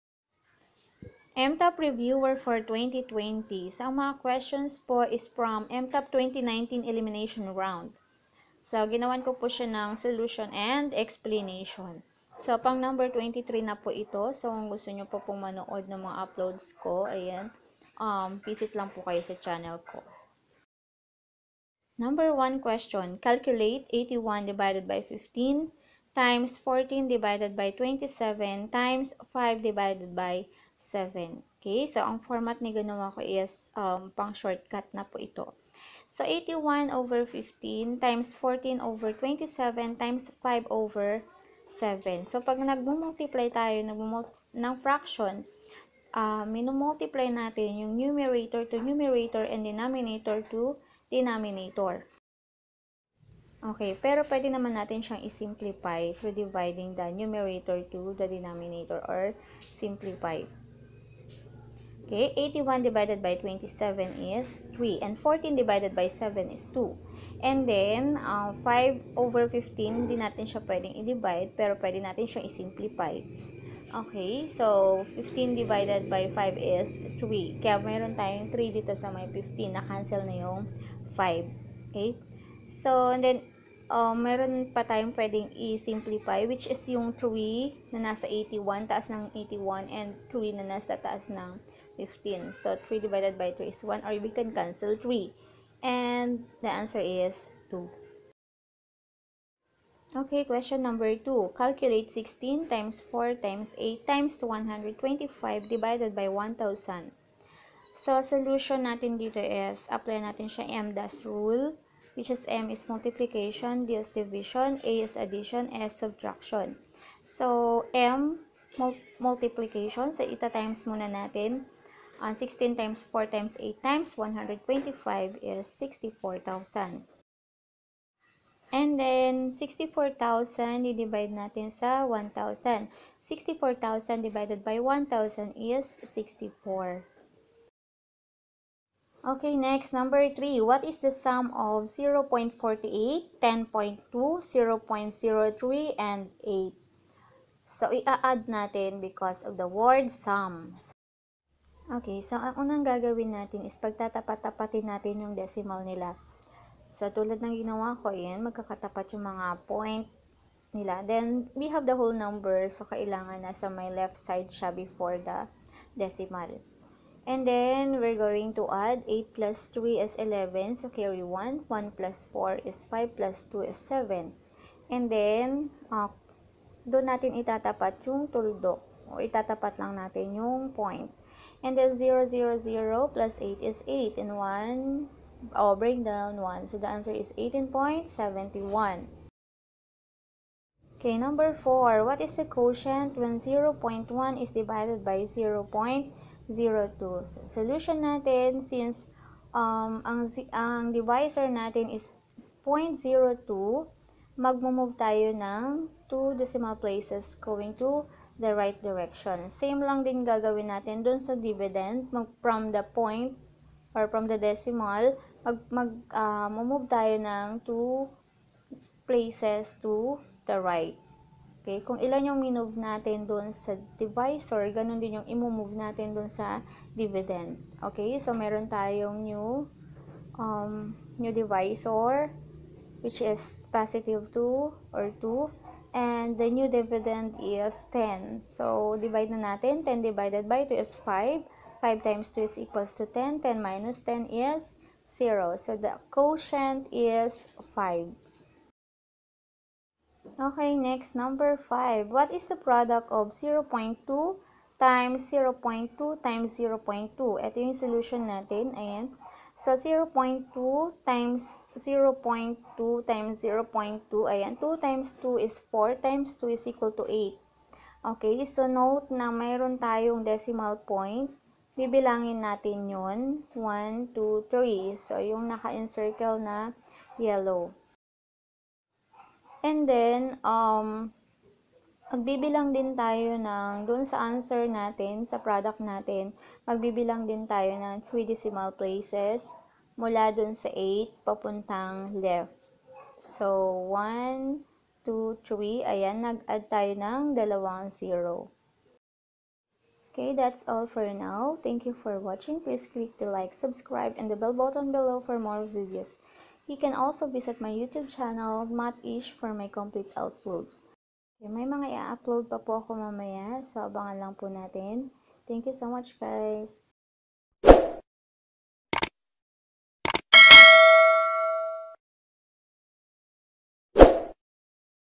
MTAP REVIEWER :#23 ADDING, MULTIPLYING AND DIVIDING DECIMALS | FRACTIONS | APPLYING MDAS RULE
Questions from MTAP 2019 elimination round.